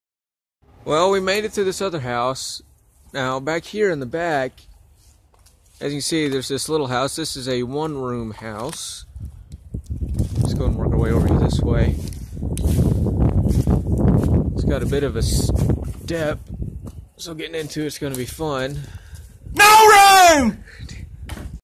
In God with an OLD CREEPY HOME PLACE - One Room House
Just a clean funny joke. Get out and enjoy what God has done In Natures Great Out Doors and see what OLD HOME PLACE you can find today. I Pray May God Bless You